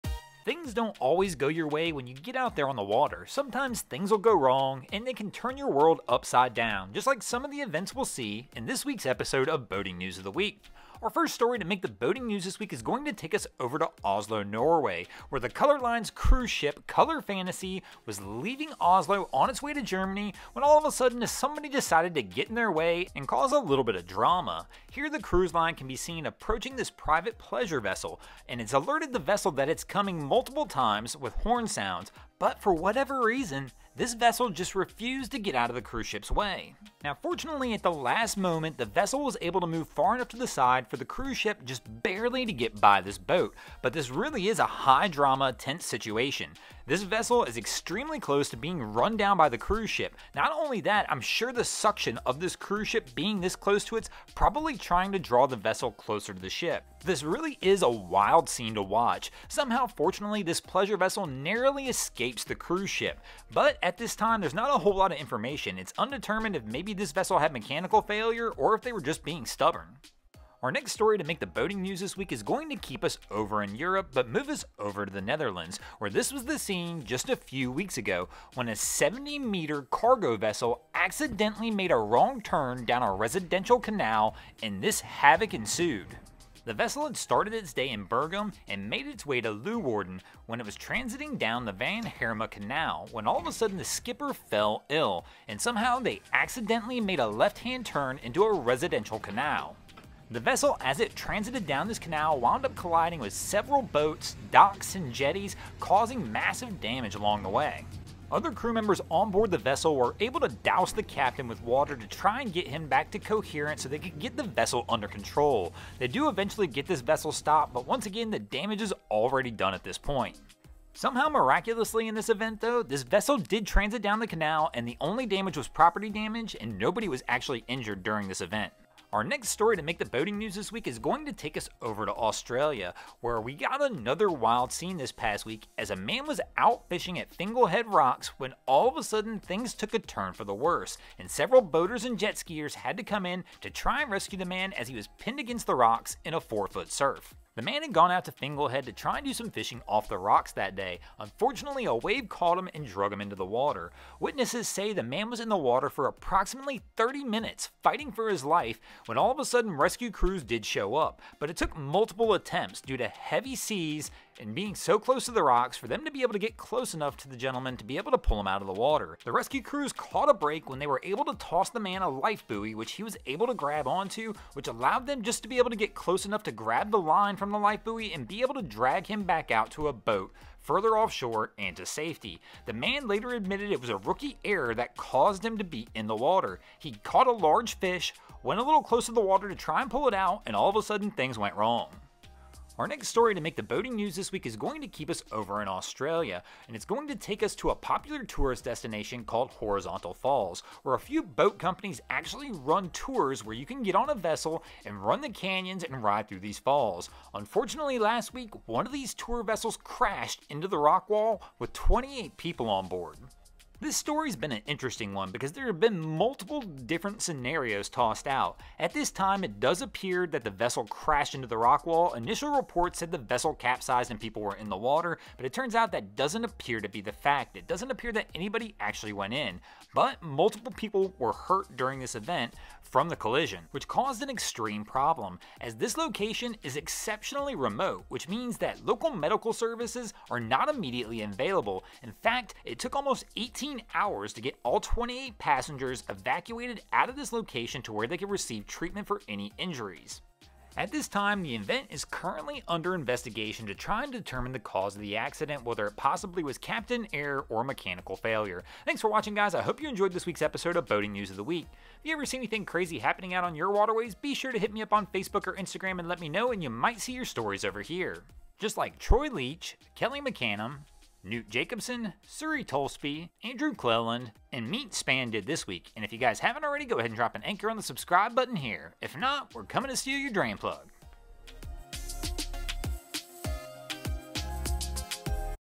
Things Go Wrong From Here!! | Boating News of the Week | Broncos Guru
Welcome back to Boating News of the Week!  As usual we review some clips of some people doing some silly things on a boat mixed in with some people doing incredible things on boats.

This week we have a wild one as an Oil Tanker takes on a wall!!

We also started a new boating group on FB called Drain Plug Mafia be sure to join to participate in everything boating.